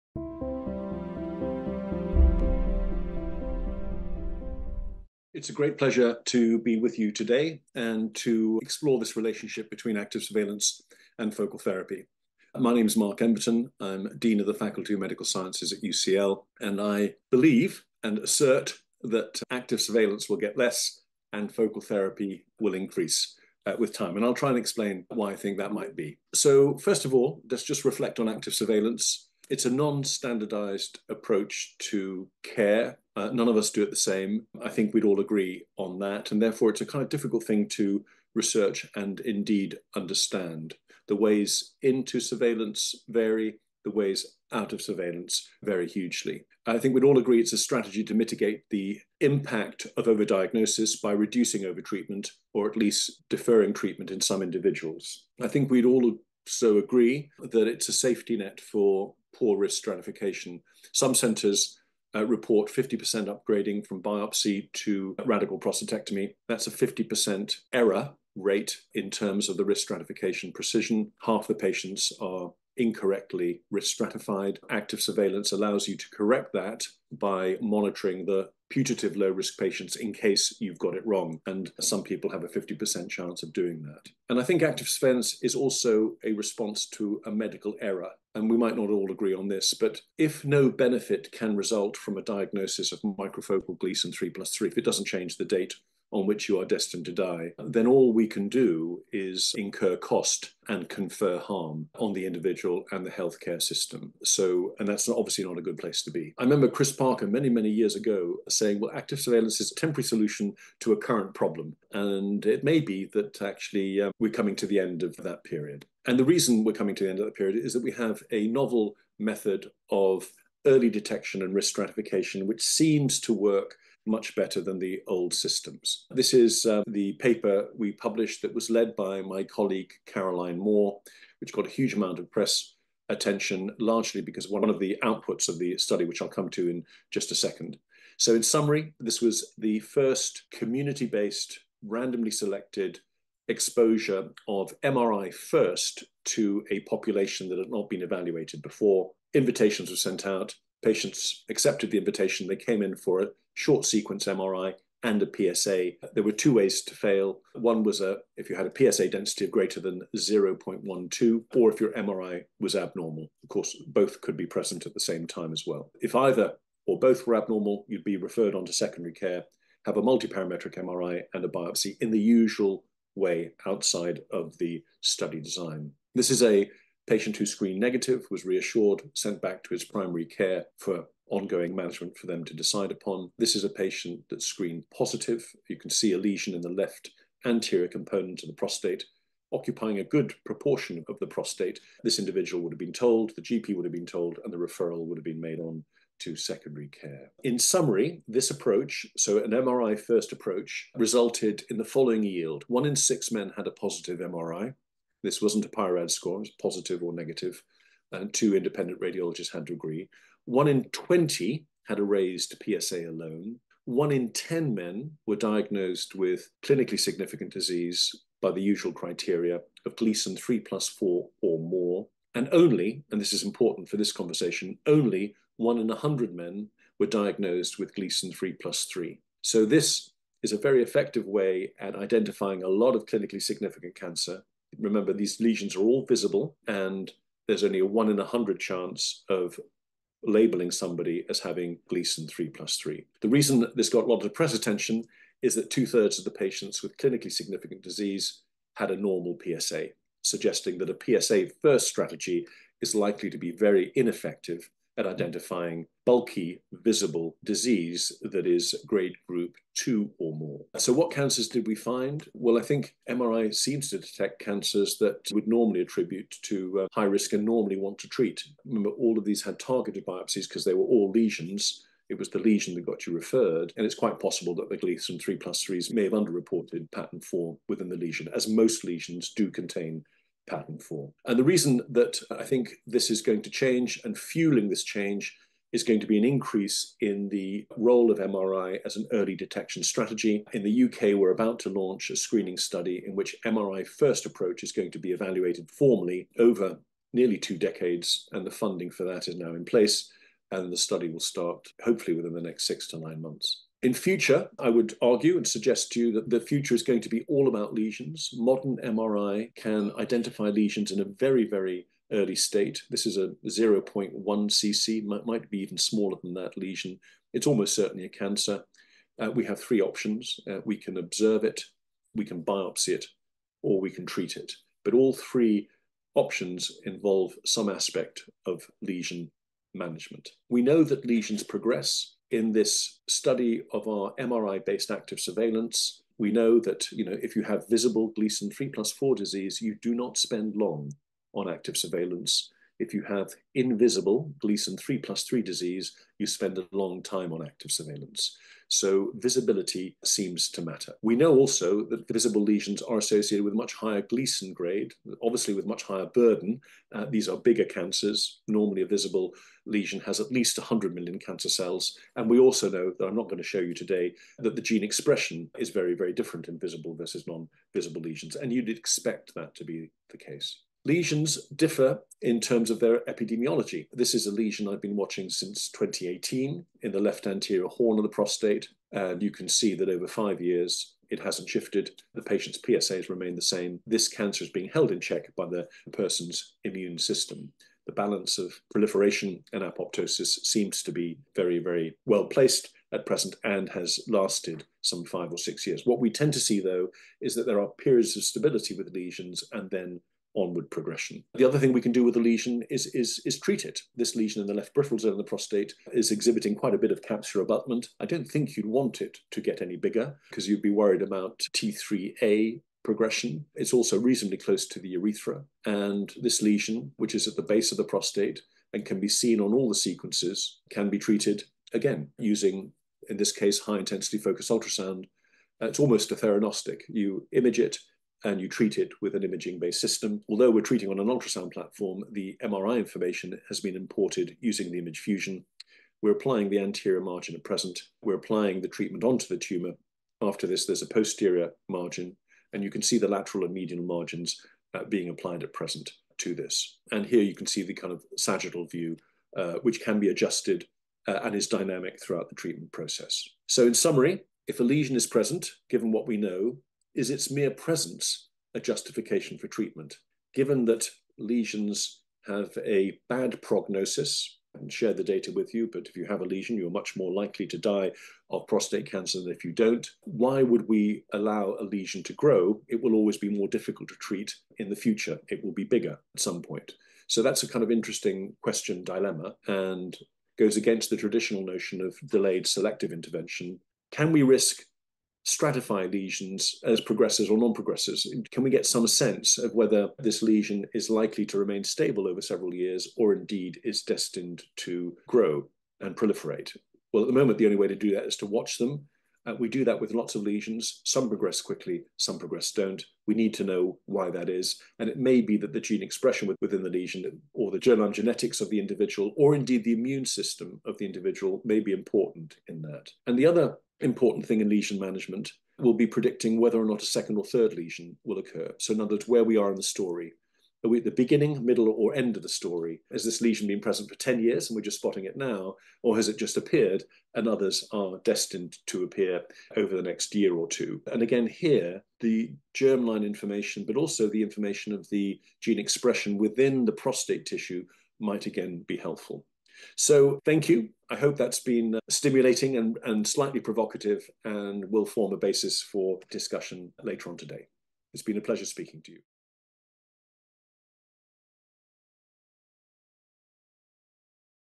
Changing Relationship Between Active Surveillance and Focal Therapy with Dr. Mark Emberton, MD, FRCS
Mark Emberton, MD, Dean of the Faculty of Medical Sciences at UCL, presents an insightful discussion on the evolving roles of active surveillance and focal therapy in managing prostate cancer. This engaging presentation spans approximately 10 minutes and offers a provocative perspective on the future of prostate cancer management.
Dr. Emberton suggests that active surveillance, historically used to mitigate overdiagnosis and overtreatment, will decrease as focal therapy emerges as a preferred strategy. He highlights the transformative potential of novel imaging techniques like MRI in early detection and risk stratification, referencing a pivotal study demonstrating that an MRI-first approach effectively identifies clinically significant cancers. Remarkably, two-thirds of clinically significant cancers were detected in men with normal PSA levels, underscoring the limitations of PSA-first strategies.
Emberton emphasizes the critical role of lesions in future prostate cancer management. Emerging therapies, such as high-intensity focused ultrasound (HIFU), allow precise, imaging-guided treatment of lesions, merging diagnostic and therapeutic approaches. Dr. Emberton envisions a shift toward lesion-centric management, incorporating genetic, immune, and molecular profiling to predict progression and recurrence.